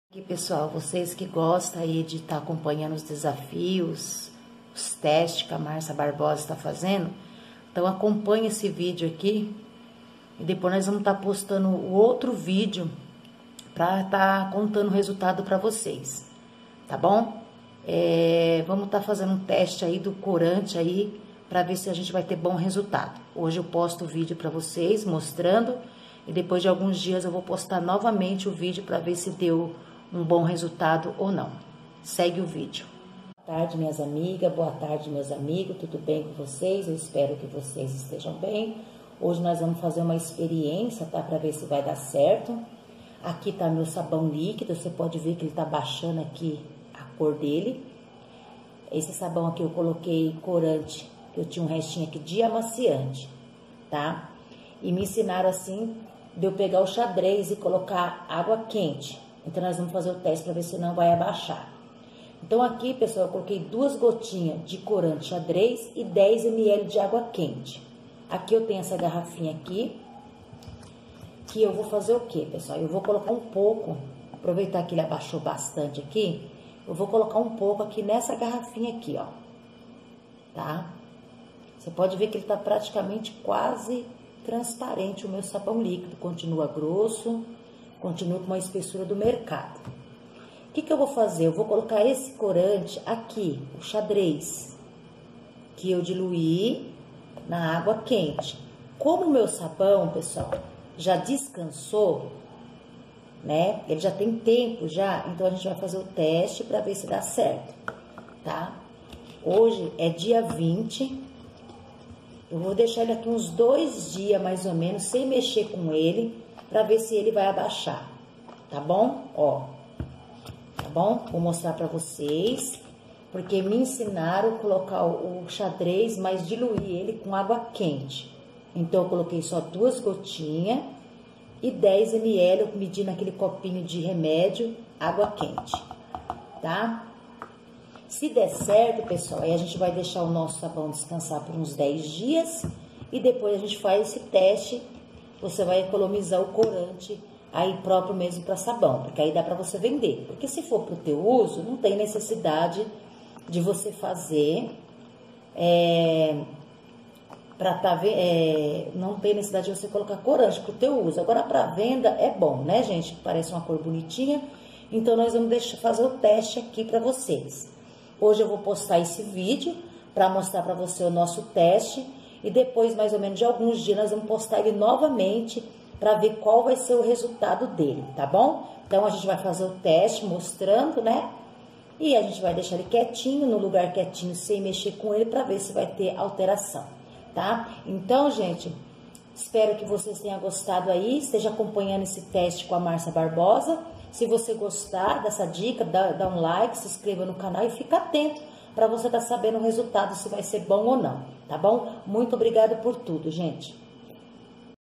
Acompanhe a Márcia fazendo teste do corante para vê se dá certo para nosso sabão caseiro
Prazo de alguns dias eu volto a mostrar o resultado ,espero q de certo agradeço o apoio de todos ,estou fazendo o melhor pra ajudar cada um de vcs
Esteja apoiando meu canal
E vcs estão em minha orações
É uma forma de agradecer a todos 😘